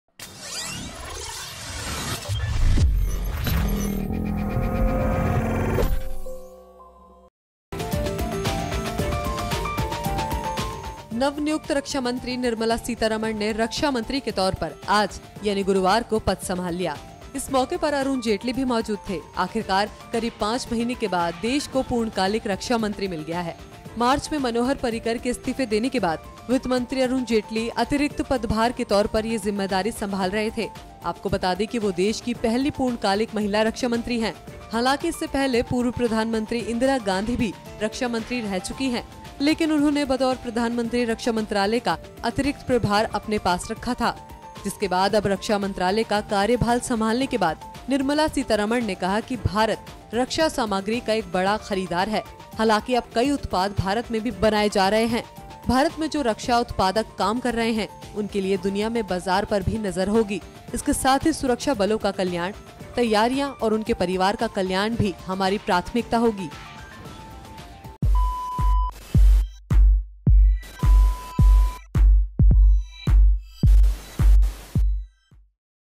Nirmala Sitharaman ने संभाला Defence Minister का कार्यभार, गिनाईं प्राथमिकताएं
Sitharaman was given charge of the ministry after the cabinet reshuffle on Sunday where she was elevated to cabinet rank. She is the country's first full-time woman defence minister. Former prime minister Indira Gandhi also held the , as additional charge.

नव नियुक्त रक्षा मंत्री निर्मला सीतारमण ने रक्षामंत्री के तौर पर आज यानी गुरुवार को पद संभाल लिया... इस मौके पर अरुण जेटली भी मौजूद थे... आखिरकार करीब 5 महीने के बाद देश को पूर्णकालिक रक्षा मंत्री मिल गया है
Producer - Sunita
Video Editor - Ajay Upadhyay